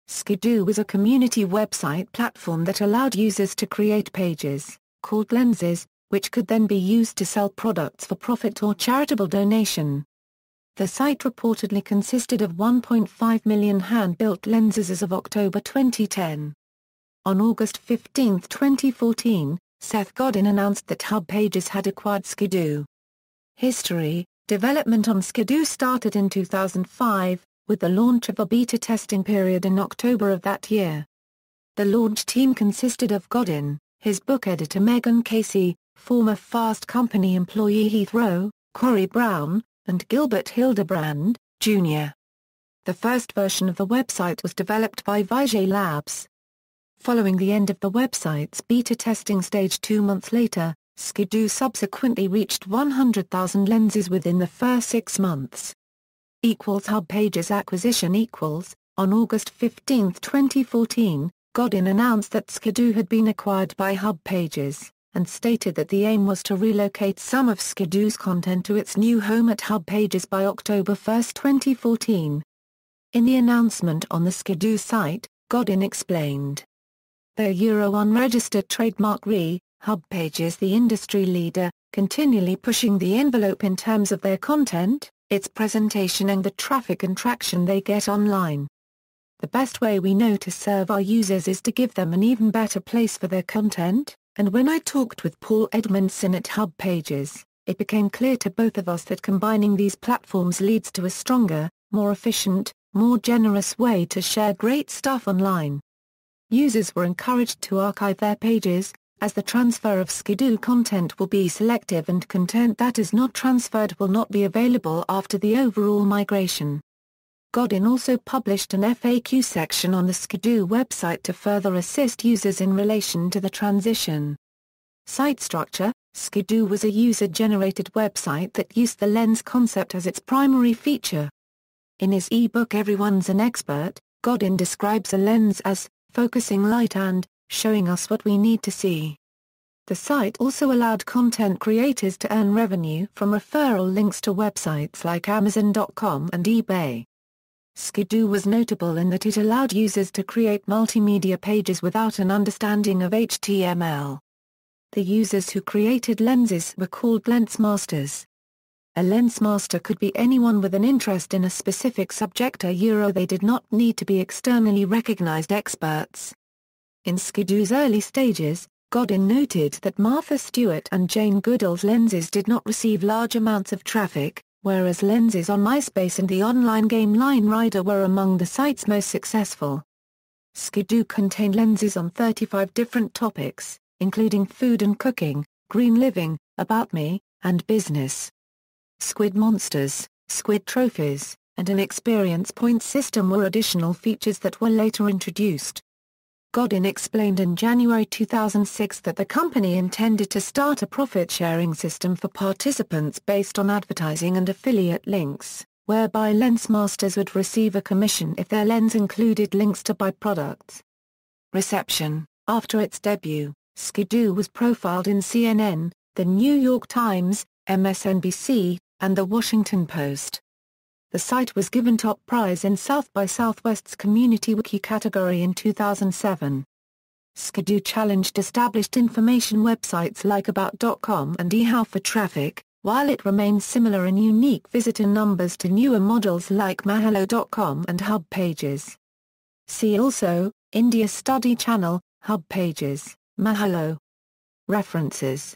Squidoo
Squidoo was a community Web site platform that allowed users to create pages, called "lenses," which could then be used to sell products for profit or charitable donation. The site reportedly consisted of 1.5 million "hand-built lenses" as of October 2010. On August 15, 2014, Seth Godin announced that HubPages had acquired Squidoo.